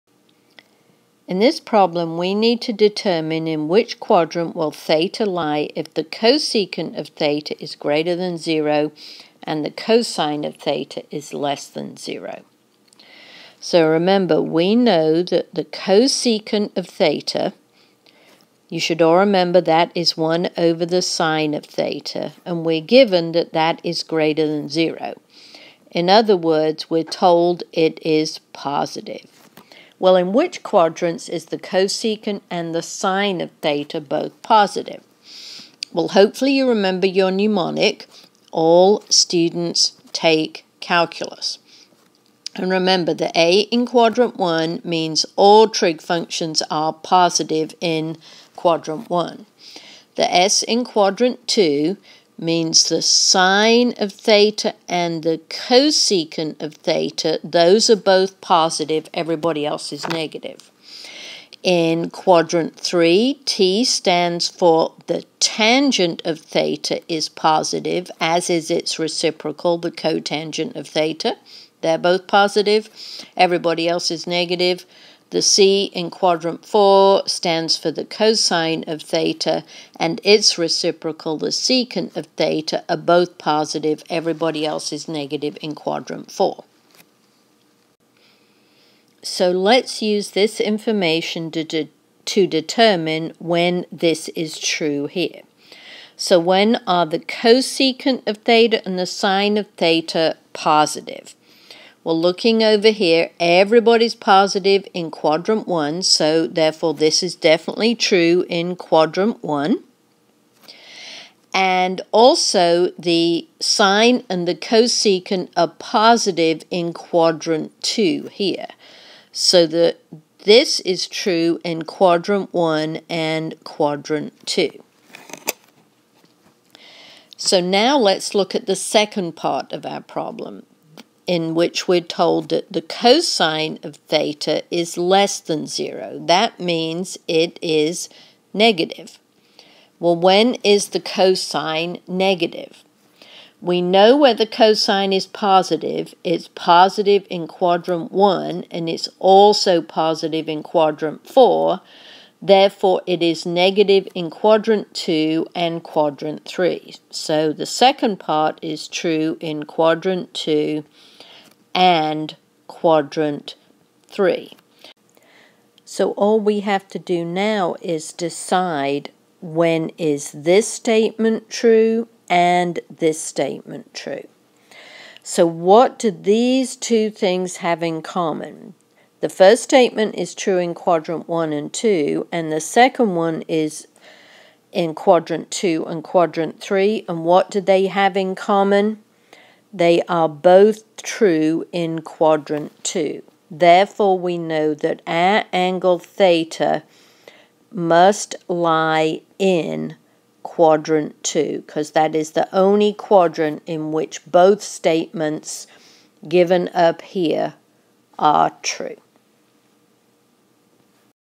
Determine the Quadrant of Terminal Side of Angle Given Signs of Two Trig Functions
Given the cosecant of angle theta is positive and the cosine of angle theta is negative, determine in which quadrant angle theta terminates.    The mnemonic "All Students Take Calculus" is explained.  The two quadrants in which the sign of the cosecant is positive are found.  Then the two quadrants in which the sign of the cosine is negative are found.  The quadrant that they have in common is the quadrant in which angle theta terminates.

Timestamps
0:00 Introduction
0:38 Mnemonic "All Students Take Calculus"
1:30 csc(theta) is greater than 0
2:10 cos(theta) is less than 0